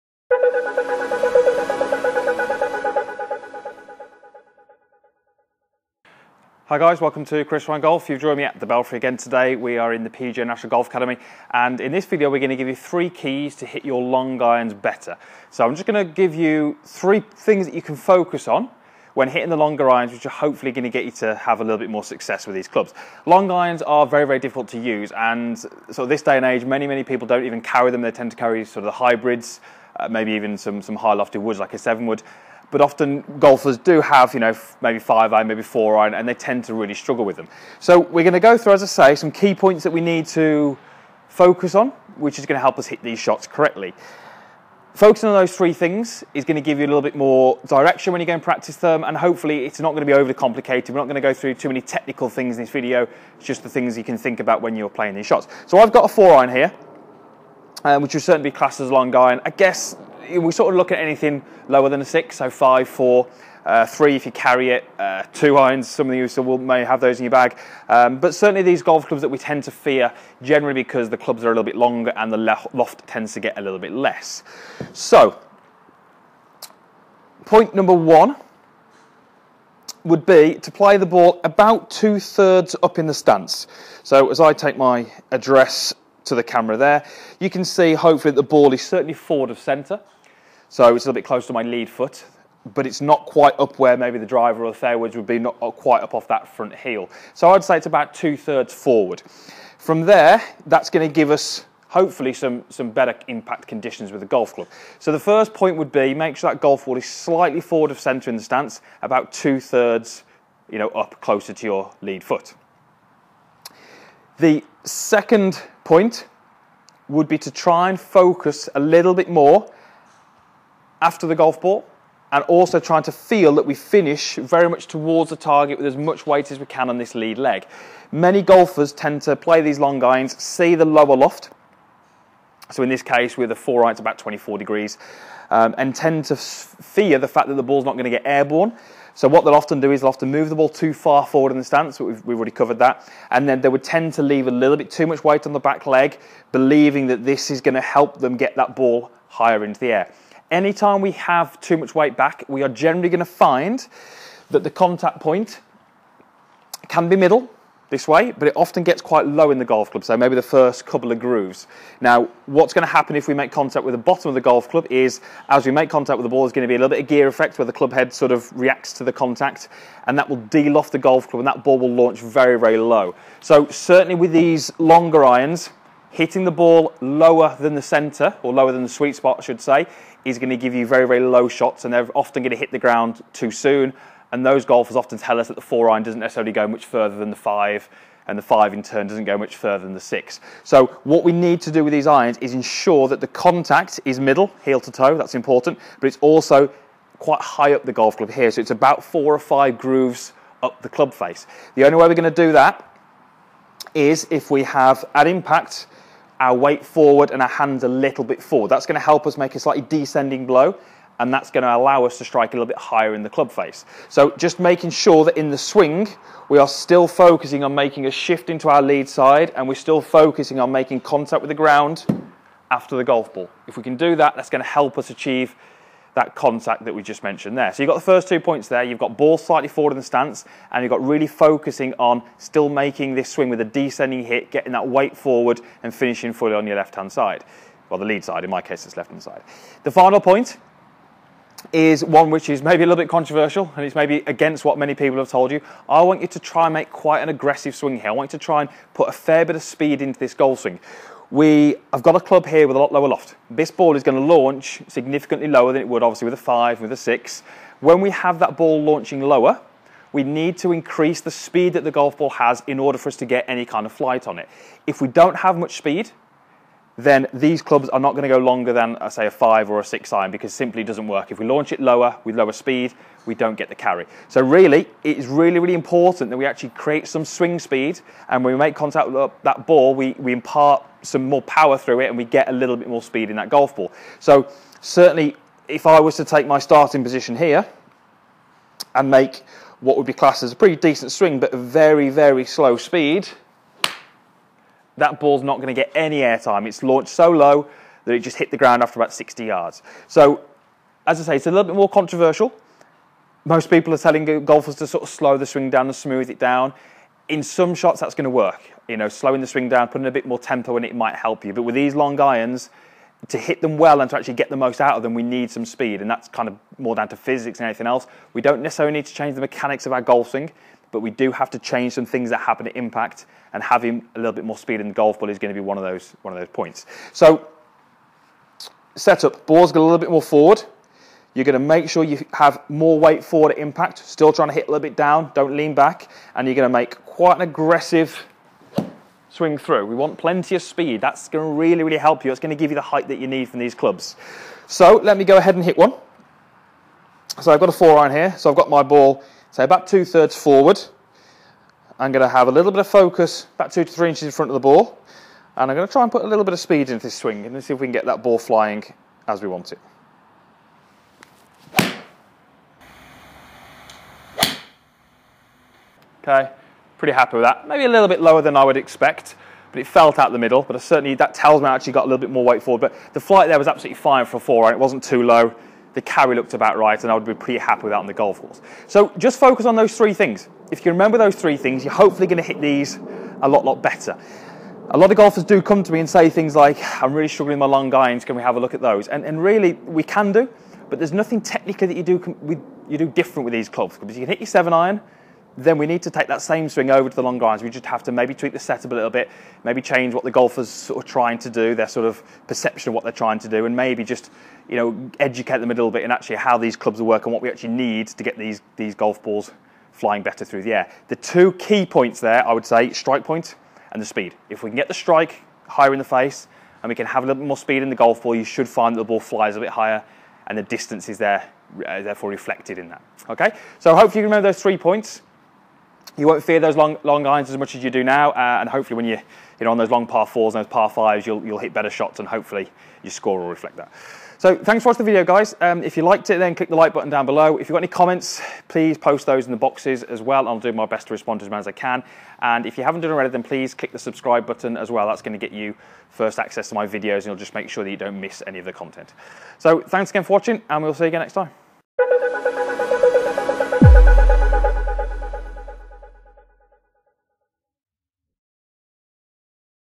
3KEYS TO BETTER LONG IRONS
As part of a new video series, Senior Coach at The Belfry Chris Ryan, gives you 3 key points that you can focus on to hit your long irons better.

Subscibe to Chris Ryan Golf for more instruction!

Golf lessons avilable with Chris at The Belfry Hotel and Resort, contact Chris for further information.